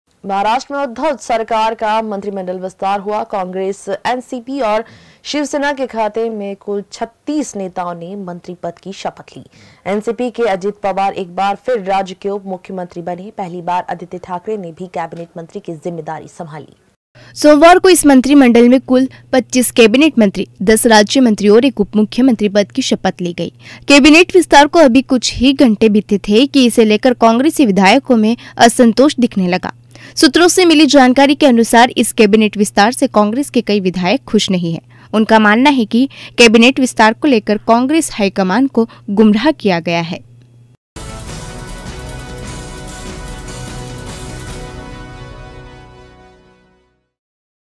MUMBAI - महाराष्ट्र में हुए कैबिनेट विस्तार
महाराष्ट्र में उद्धव सरकार का मंत्रिमंडल विस्तार हुआ. कांग्रेस, एनसीपी और शिवसेना के खाते से कुल 36 नेताओं ने मंत्री पद की शपथ ली। एनसीपी के अजित पवार एक बार फिर राज्य के उपमुख्यमंत्री बने, पहली बार आदित्य ठाकरे ने भी कैबिनेट मंत्री की जिम्मेदारी संभाली।